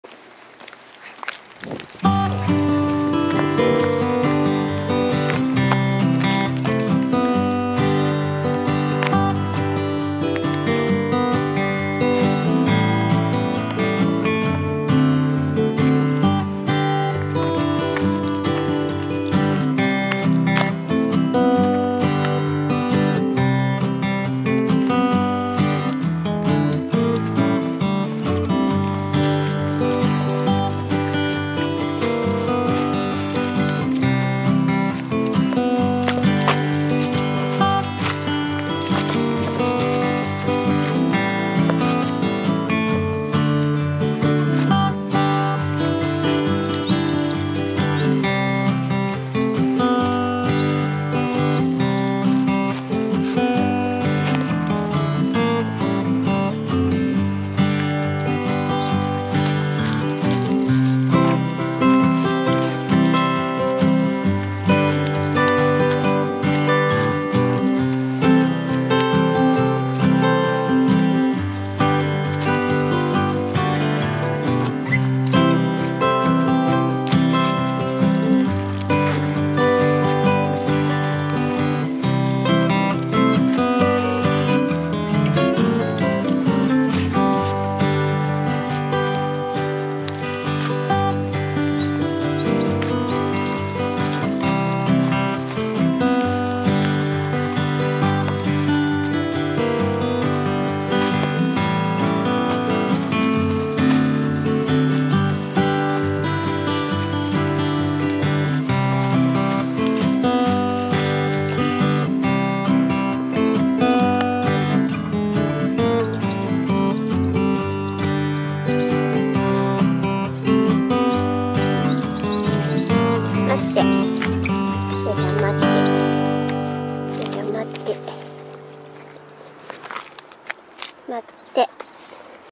3才8か月13日ドーベルマン☆散歩③田舎町と住宅地をつなぐ道にて♪☆
2020年2月3日月曜日13：59、散歩①～⑥まであります。冬の枯れた田んぼを見下ろす見晴らしの良い場所で。。リンダは、さぞ爽快だったことでしょう♪だいたい散歩時間は1時間以上、お昼ごろの時間はのんびりお散歩できます♪リンダは朝も夜も散歩に行きます♪